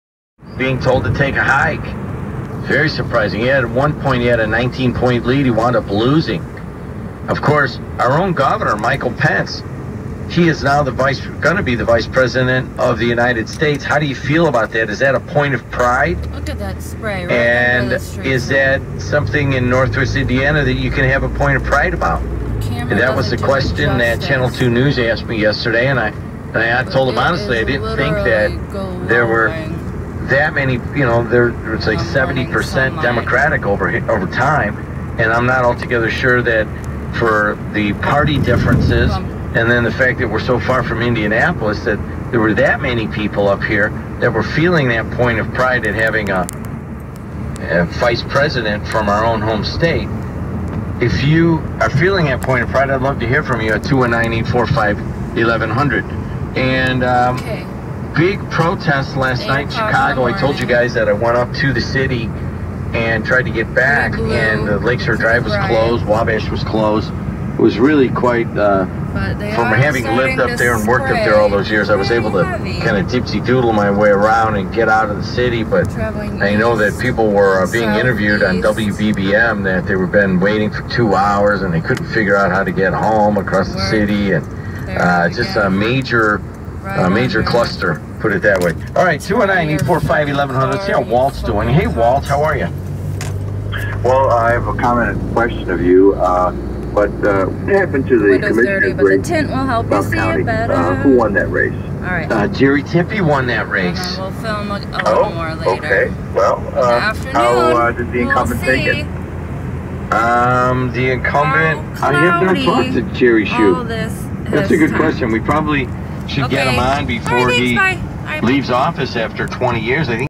Chemspray Cloud Seeding 11/10/16 7am
Another boring drive to work but i couldn't help but film the little glowing cloud